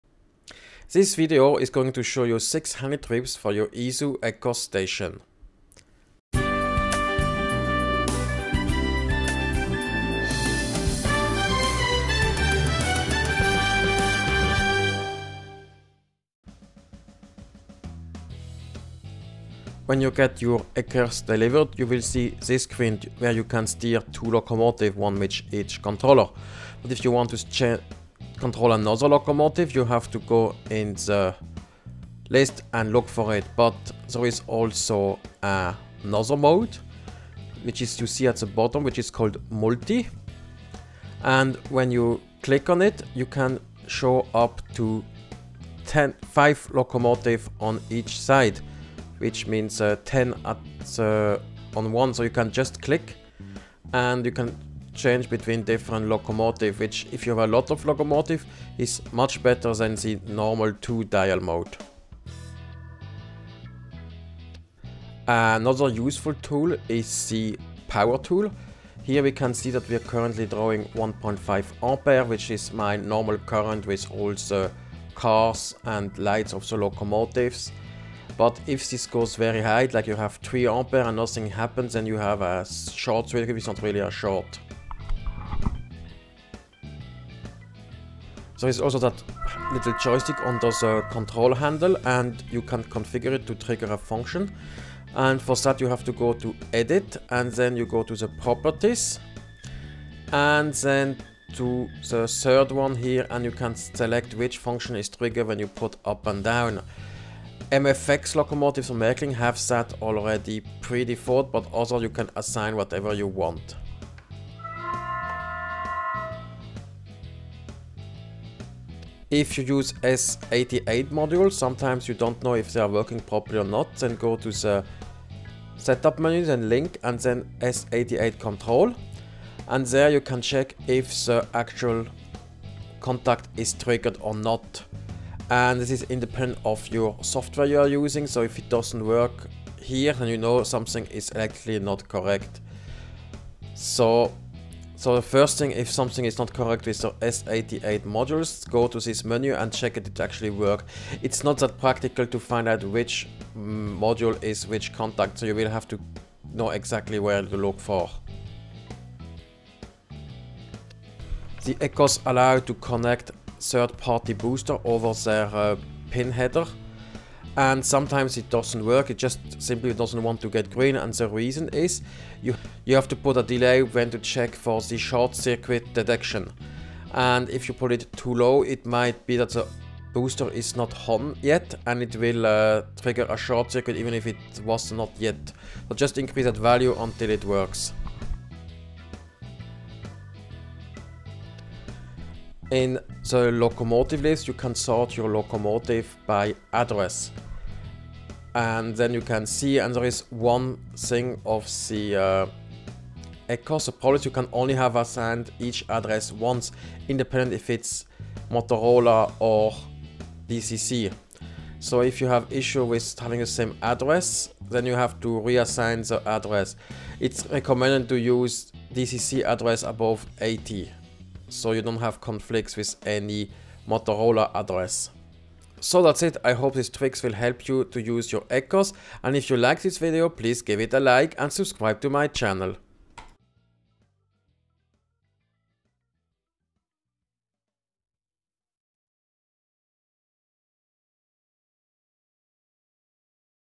6 advanced tips for your ESU ECOS
The ECOS has a lot of functionality. Here are 6 tipps how to use it better.

0:00 - Introduction
0:16 - Multiview
1:02 - Power Monitor
1:26 - Joystick
1:56 - s88 Monitor
2:39 - Booster Setup
3:09 - Addressing
3:45 - End